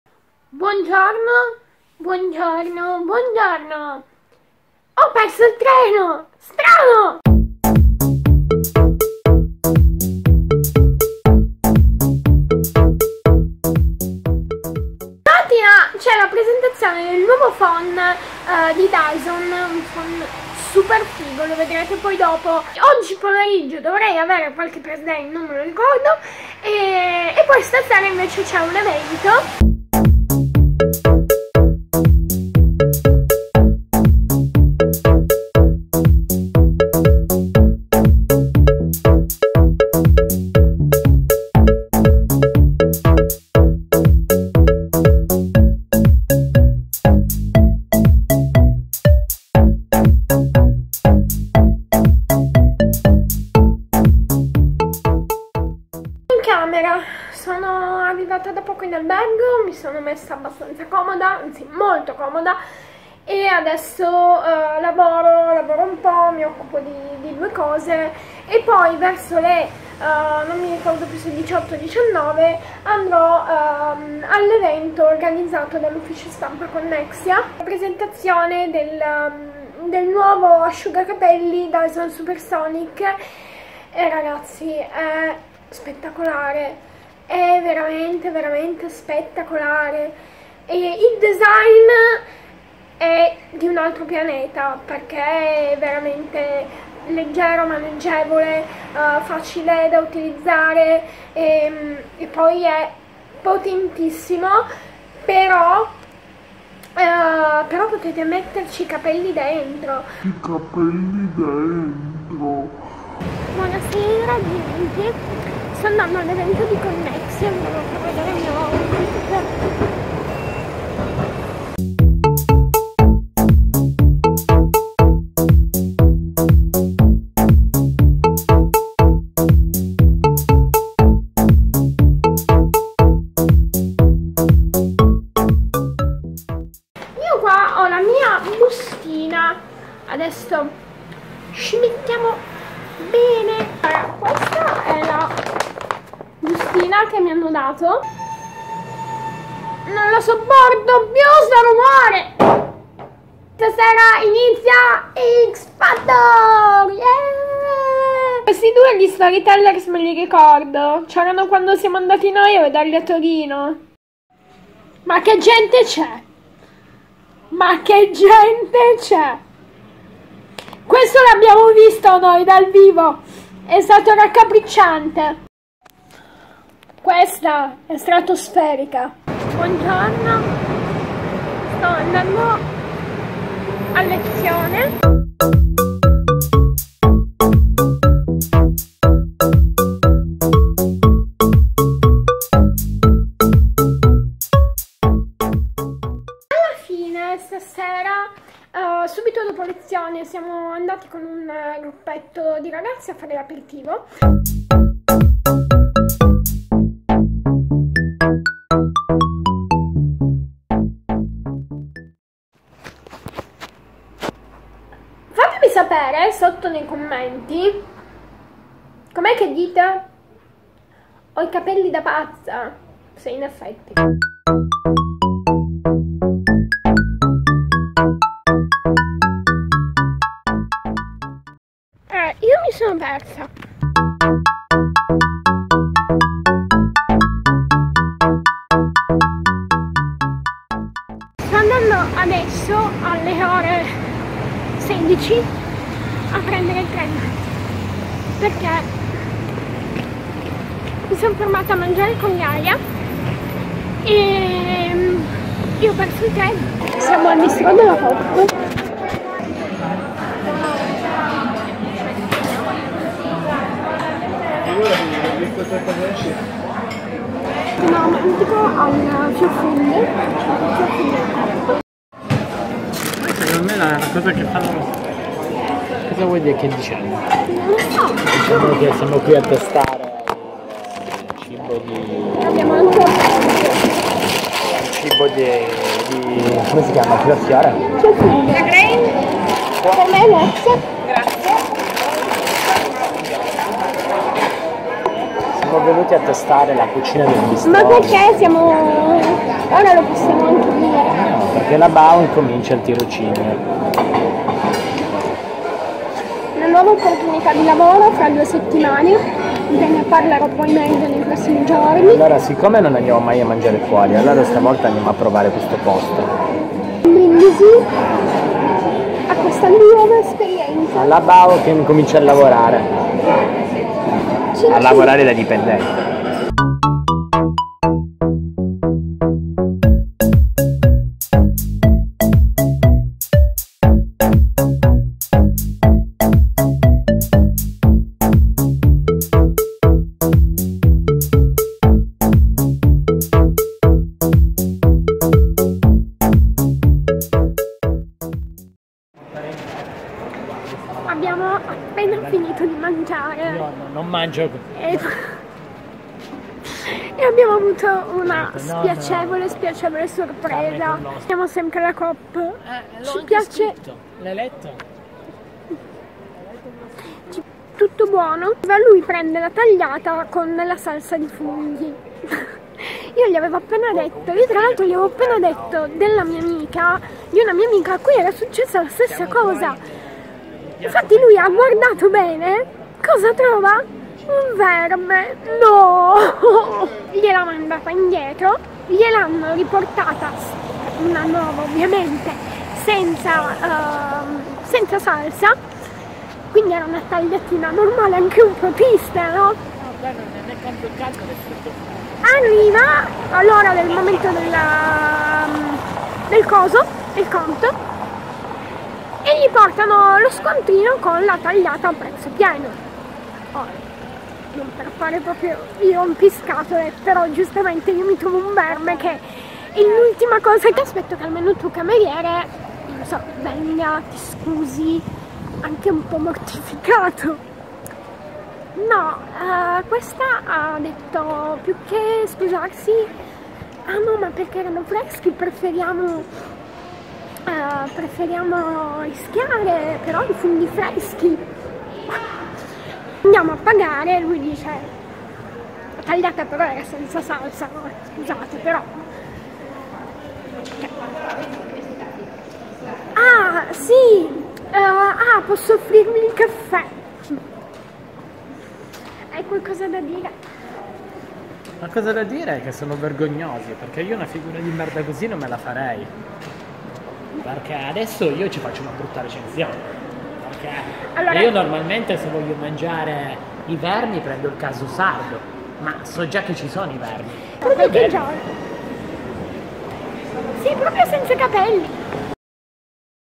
EVENTO DYSON SUPERSONIC, UN VERME NEL PIATTO! :( | SSMLIULM #9 (Vlog 15-18.09)
Vi porto con me all'evento presentazione dell'asciugacapelli Dyson Supersonic... e per finire, una cena al bistrot Fiorfood Coop a Torino con una sorpresa non molto piacevole

SEGUIMI ANCHE SU SNAPCHAT: mywaytobemyself

NOME: Ilaria Icardi